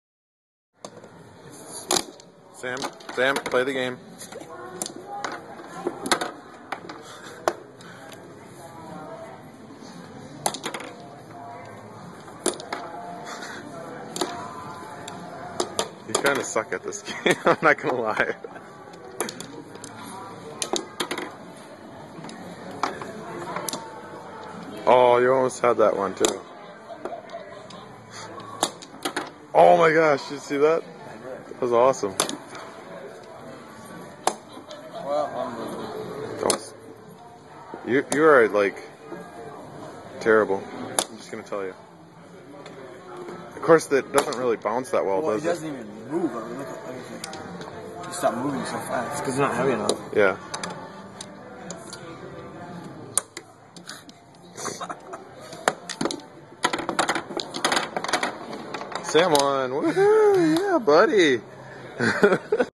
Sam playing miniature pool
Sam playing pool on a little pool table at the mall.